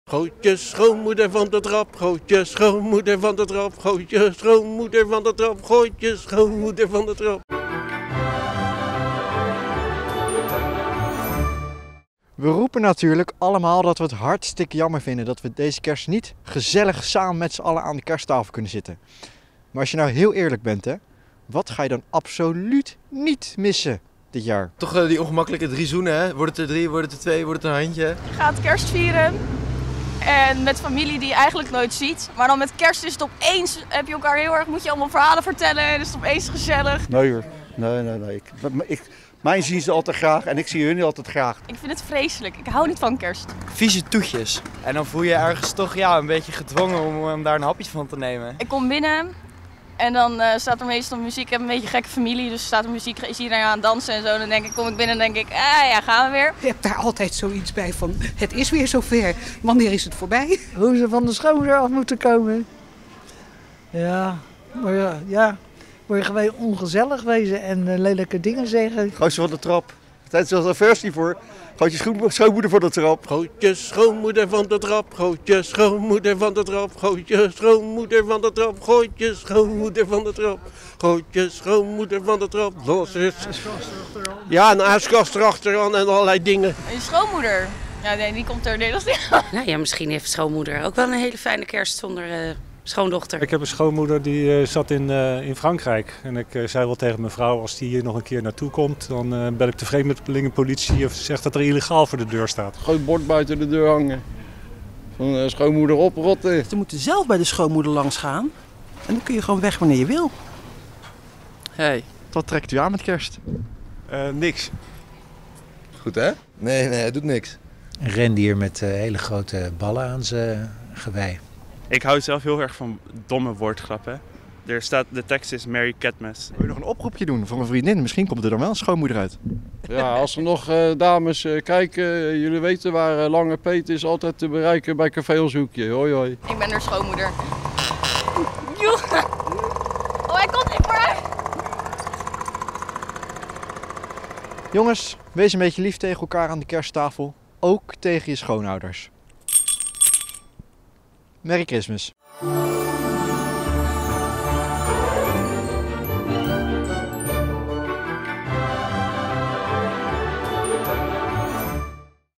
Sleutelvraag: Wat ga je niet missen met kerst?
Iedere week gaat verslaggever Pepijn van Wezenberg voor Sleutelstad de straat op om de stemming onder de Leidenaren te peilen. De vraag van deze week: Wat ga je niet missen met kerst?